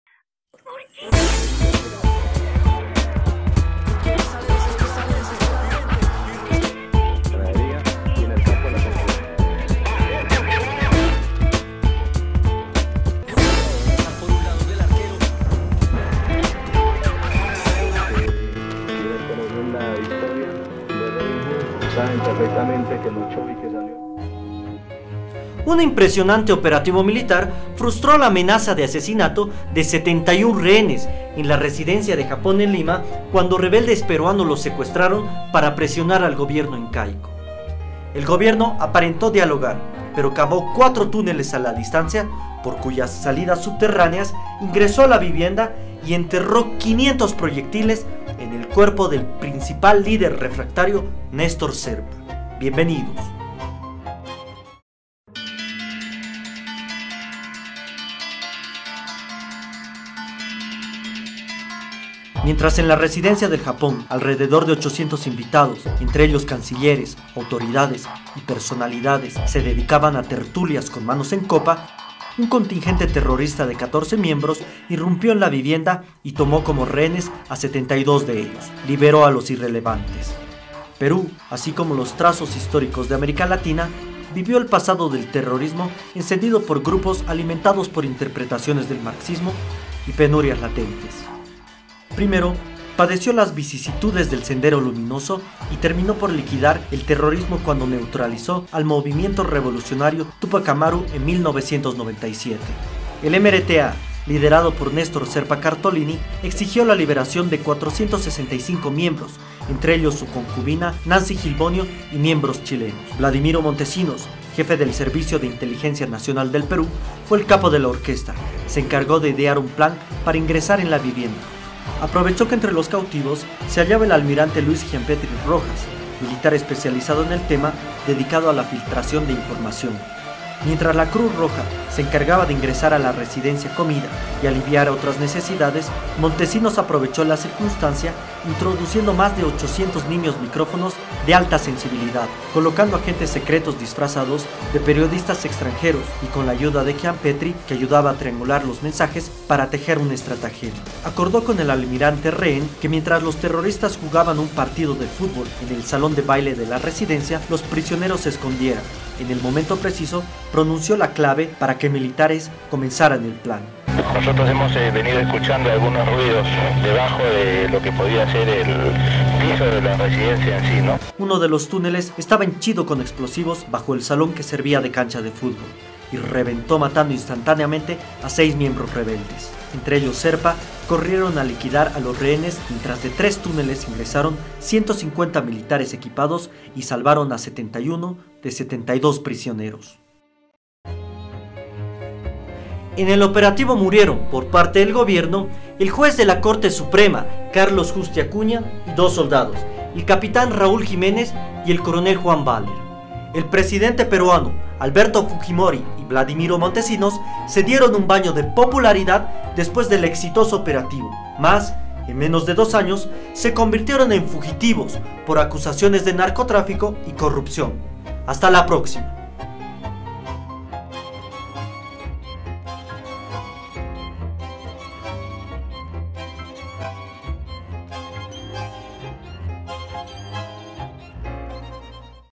Operativo Chavín de Huantar - Documentales de historia
En Perú un grupo de rebeldes tomó como rehenes a casi un centenar de diplomáticos. El gobierno aparentó dialogar pero terminó aniquilándolos con un plan militar...
Producción y dirección: Daniel Cuba Villarroel y Mario Cuba Villarroel